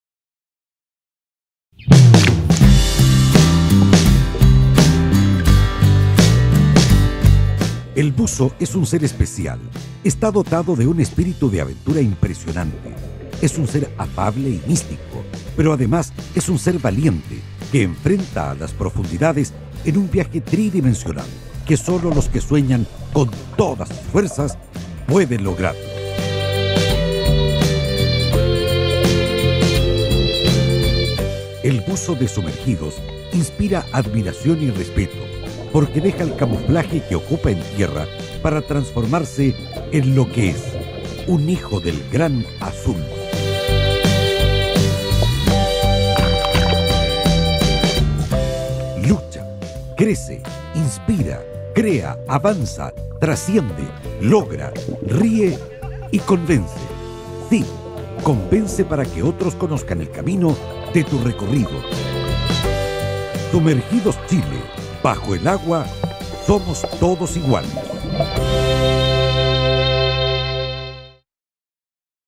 SUMERGI2 CHILE, Bajo el agua somos todos iguales
Sumergi2 Chile, team de Buceo, sin fines de lucro, destinado a buzos que se atreven a desafiar sus capacidades al máximo, en un ambiente libre, inclusivo, ingrávido y que aportara innumerables beneficios, siendo el esparcimiento y la felicidad lo primordial. Director Fundación Sr. Luis Torres Molina, Buzo Profesional; Subdirector Sr. Jorge Velásquez Andrade, Finanzas Sra. Paula Pinto.